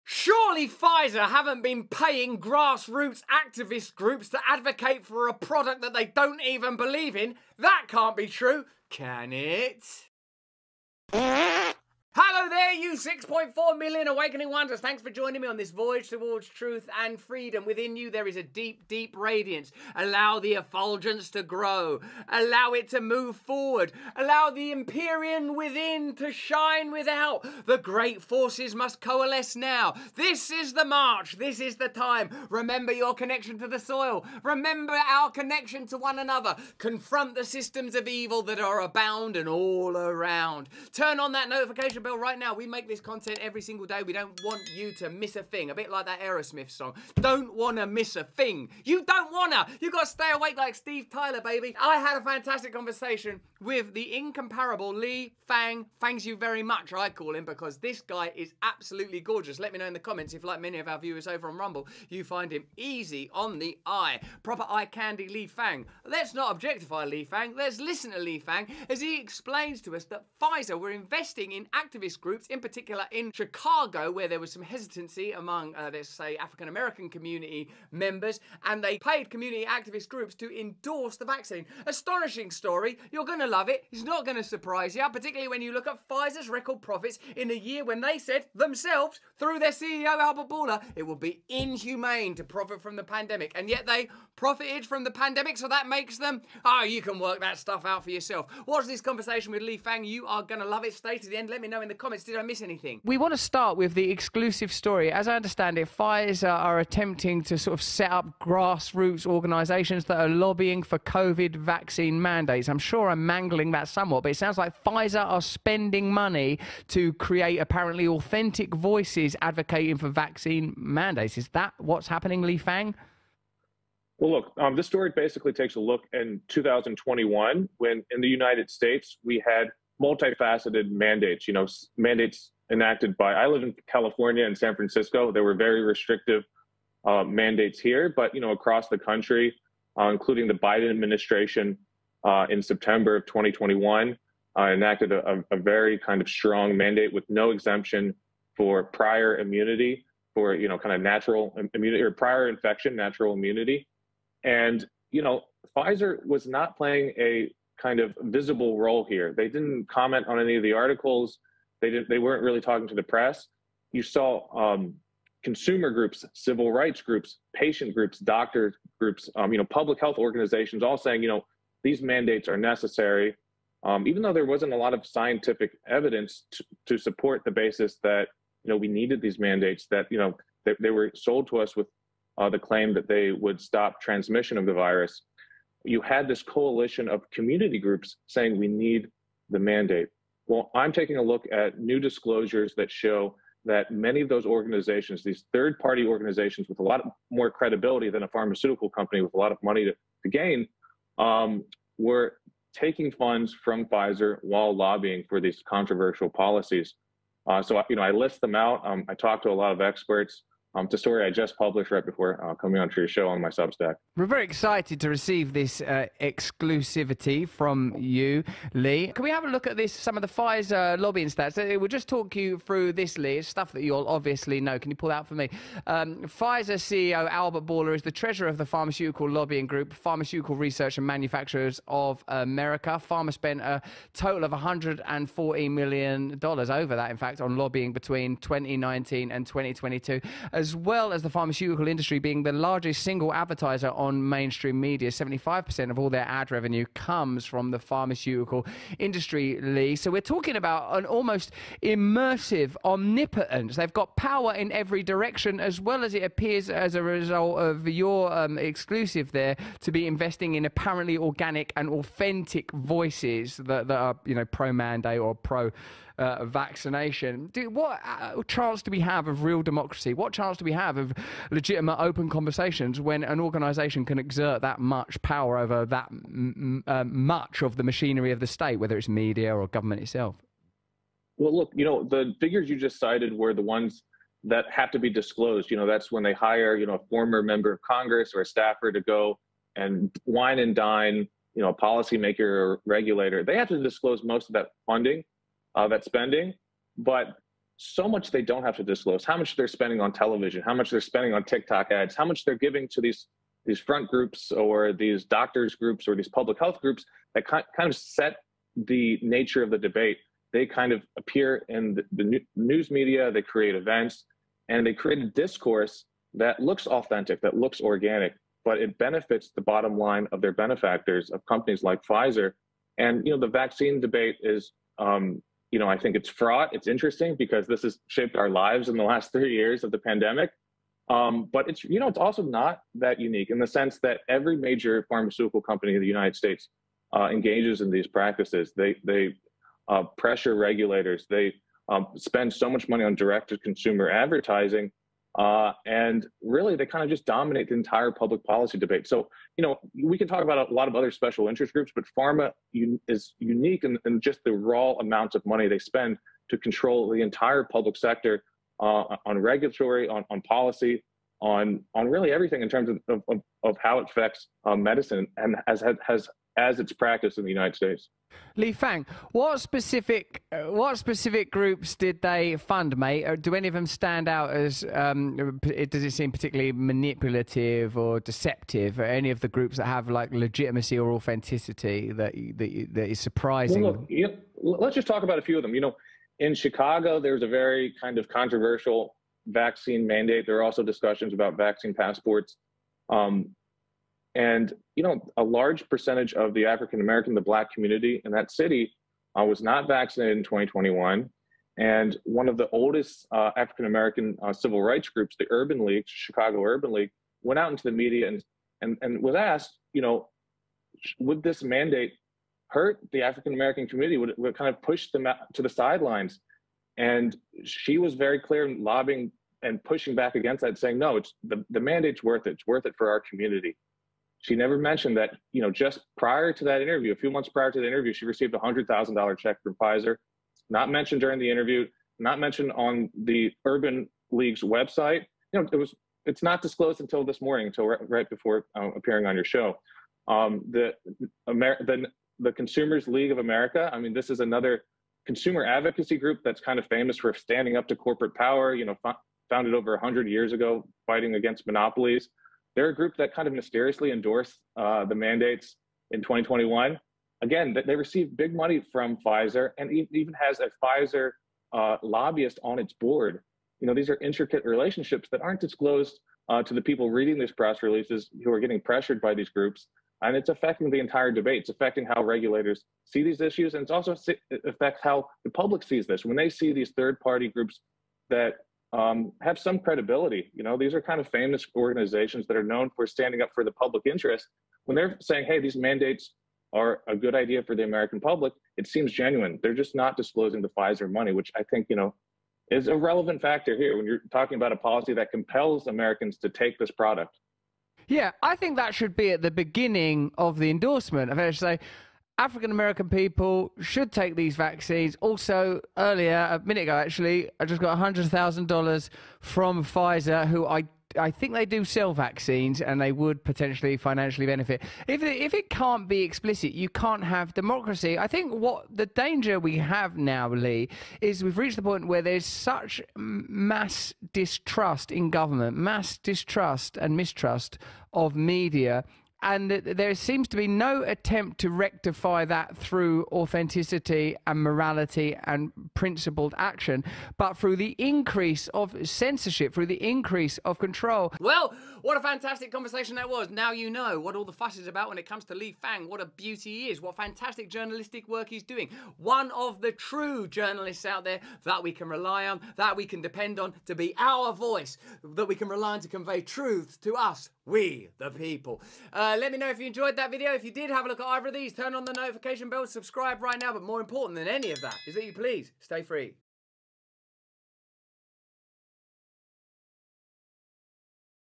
THIS Just EXPOSED Pfizer | Lee Fang’s Exclusive BOMBSHELL Report
I recently spoke with investigative journalist and heart-throb, Lee Fang! Lee shared an exclusive exposé on Pfizer - and the funding sources behind supposedly independent groups who have been pushing for vaccine mandates.